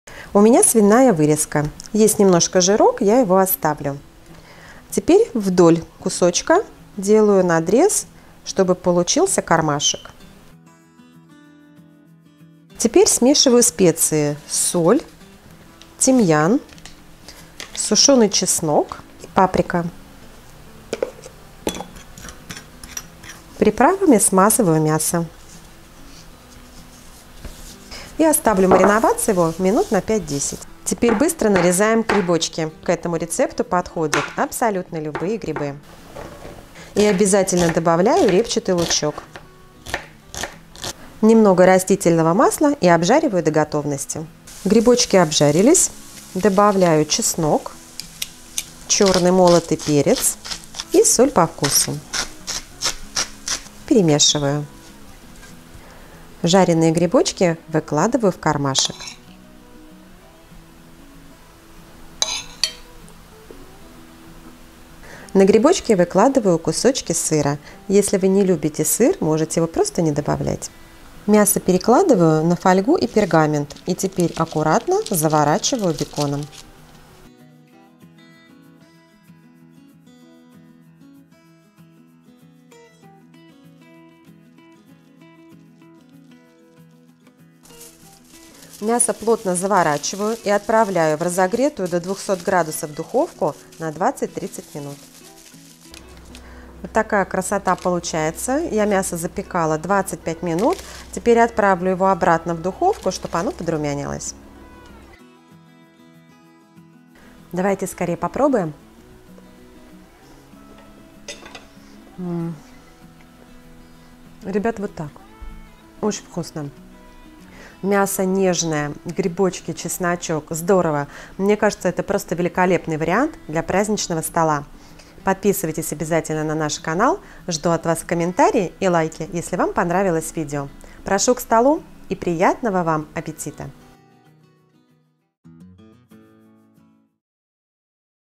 Сказочное Мясо на Новый Год! Все Будут Требовать Добавки!
Мясо на праздничный стол!
Ингредиенты:
600-700г вырезка
1ч.л сушеный чеснок
1ч.л сушеная паприка
1ч.л тимьян
соль по вкусу
200г грибы
2шт. лук репчатый
2 зубчика чеснока, перец
4-5 кусочков сыра
8-10 полосок копченого бекона